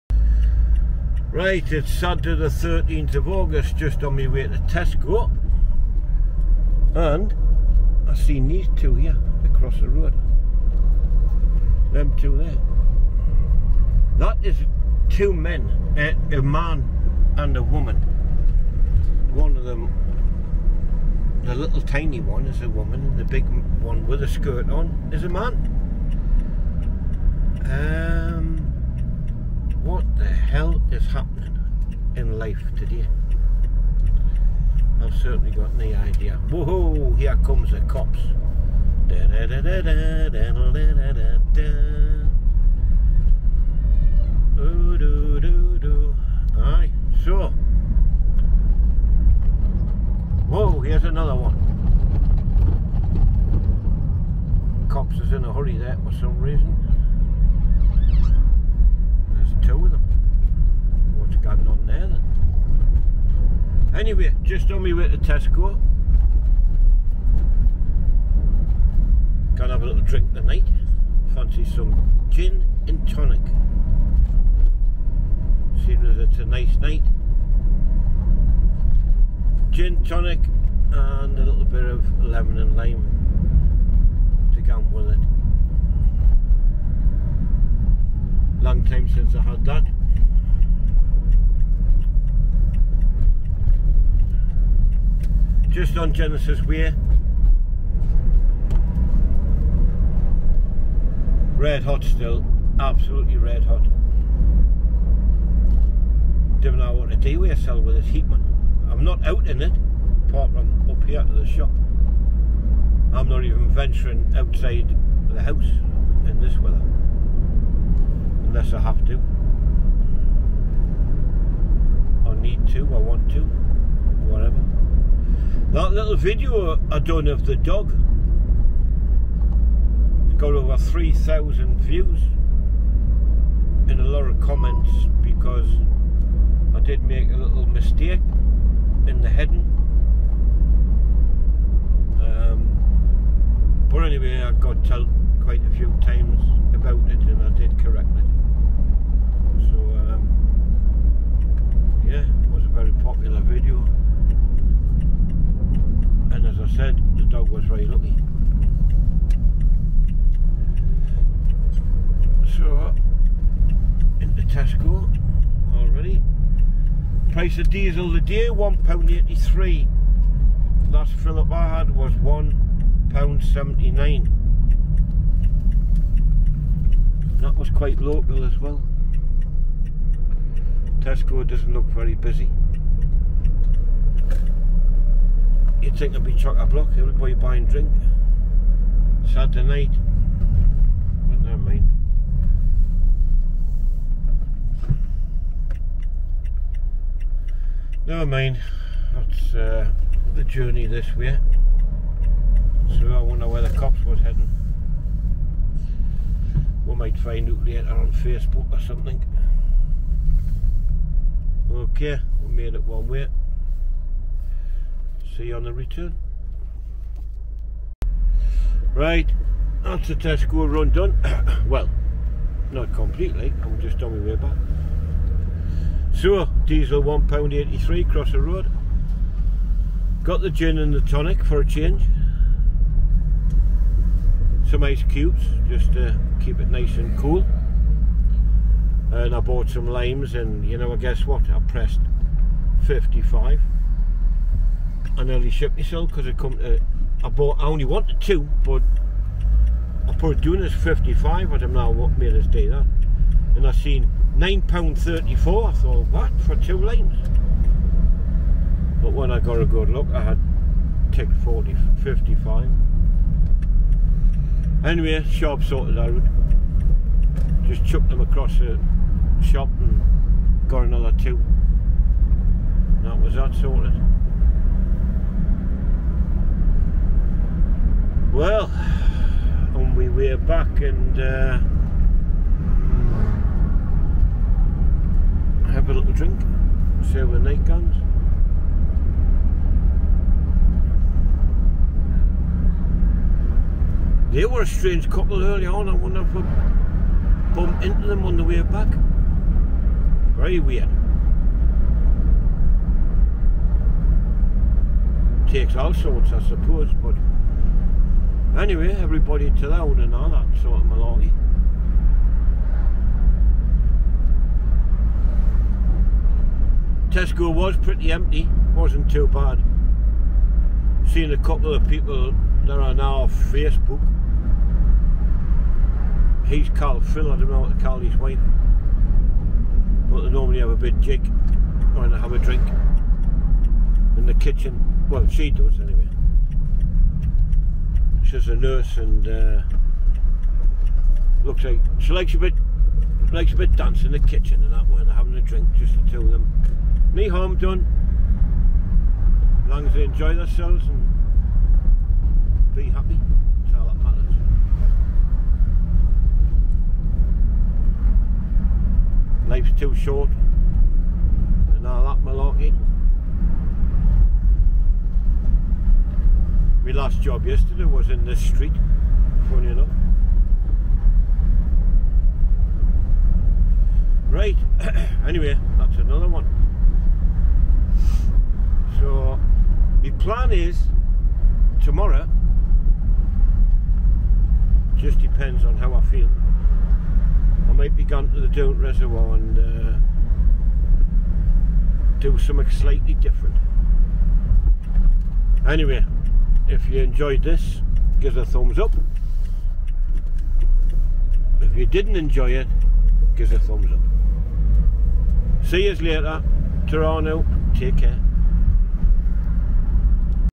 Durham police in a hurry while on my way to tesco
Weird kinda video tonight on my tesco run